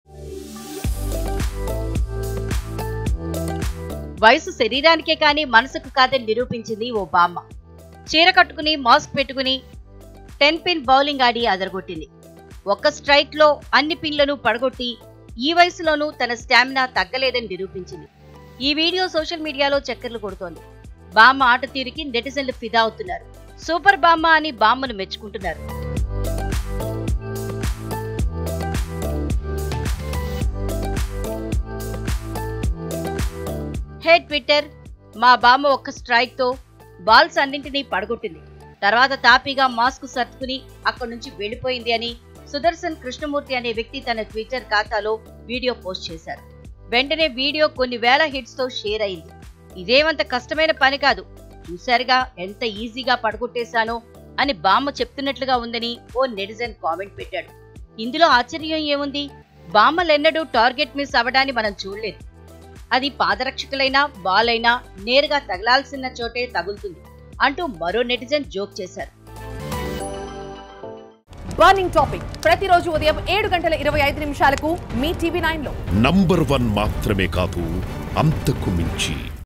Grandma bowls a perfect strike in her saree: Viral Video - TV9
Grandma bowls a perfect strike in her saree: Viral Video

Credit: NewsUpdates | Prasad/Producer